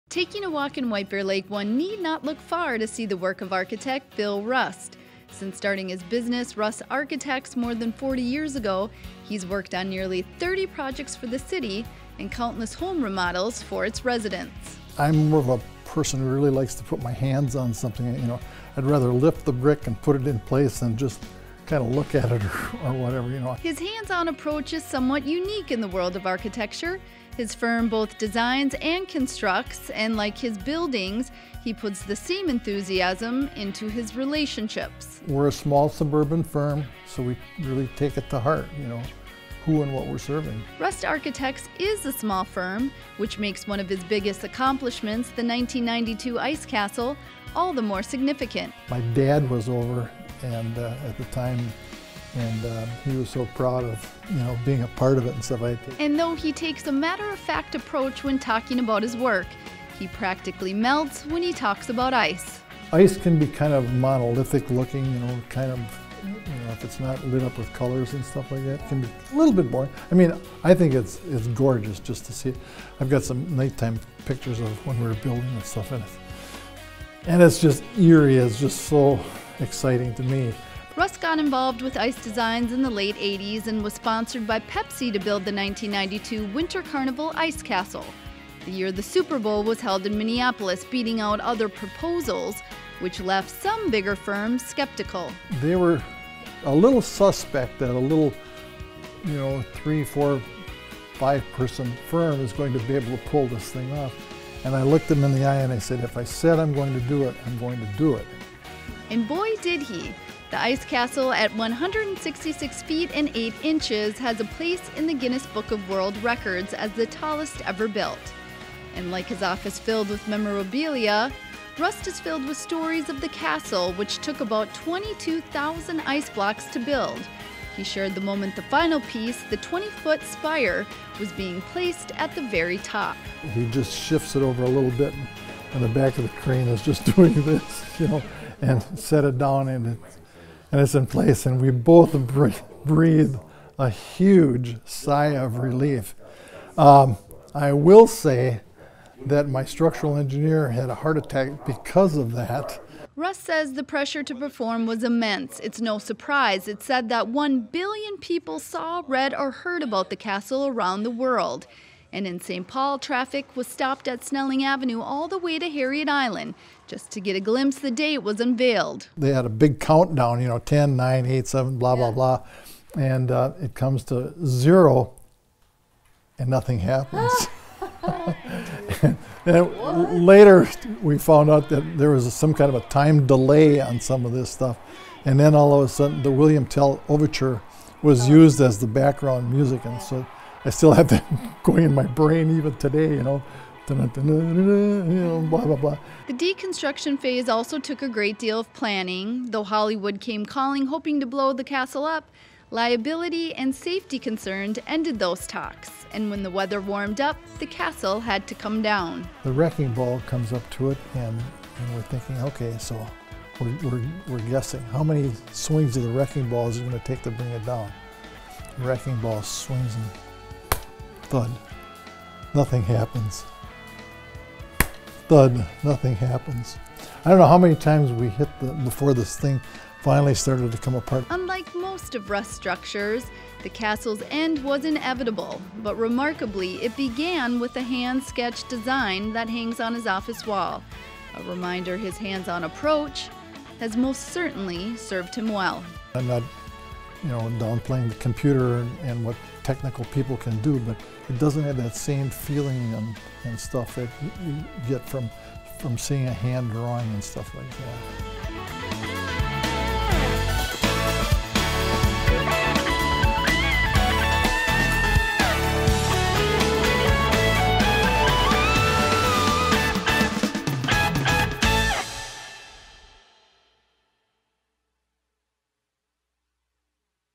1992 Winter Carnival Ice Palace
White Bear Lake architect, Bill Rust, talks about his design of the tallest ice castle that was seen around the world.